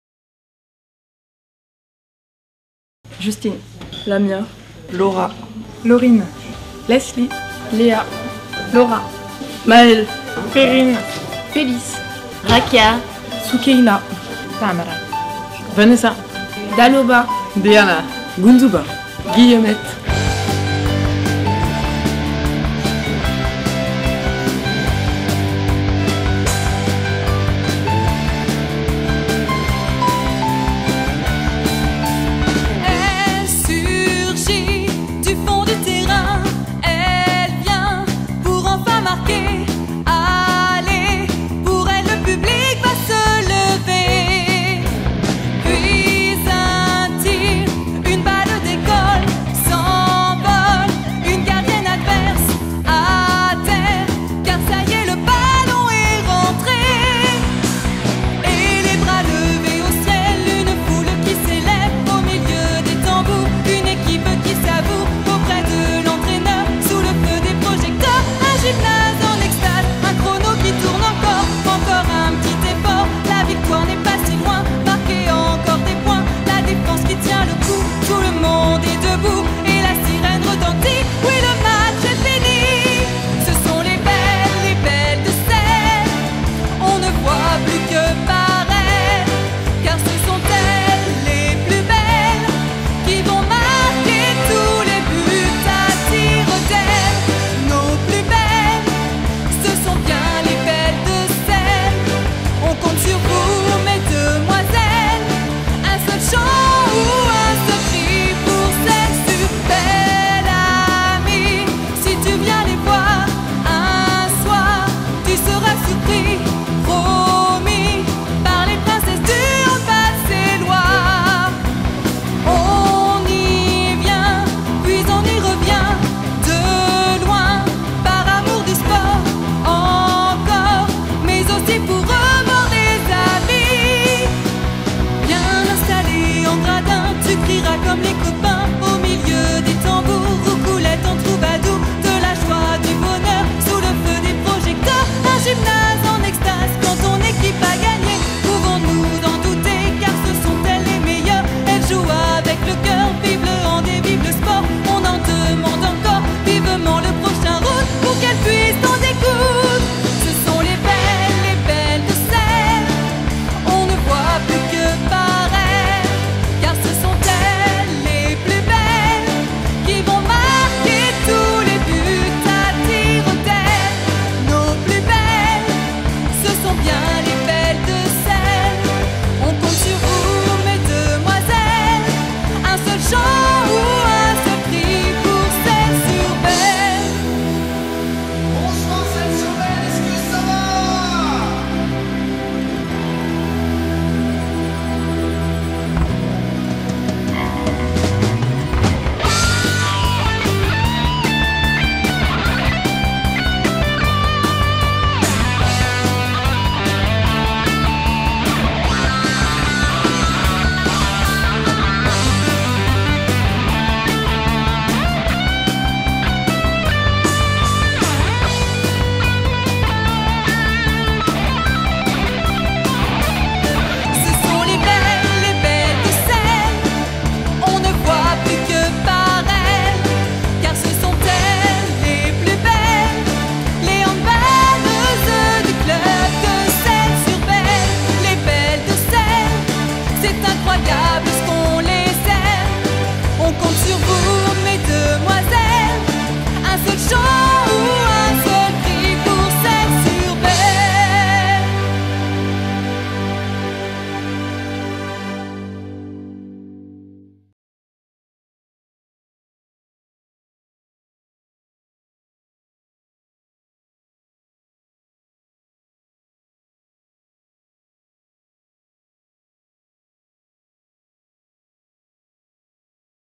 HBCC 2019 2020 LE CLIP de l'équipe D2F
Handball-Club de Celles-sur-Belle (Deux-Sèvres), le clip de la saison 2019/2020 de l'équipe élite féminine qui évolue en D2F avec le statut VAP (voie d'accession au professionnalisme) permettant d'accéder au Championnat de France féminin de handball, dénommé Ligue Butagaz Énergie ou Division 1, compétition qui représente en France le sommet de la hiérarchie du handball féminin.
Une réalisation vidéo CaptAction79 septembre 2019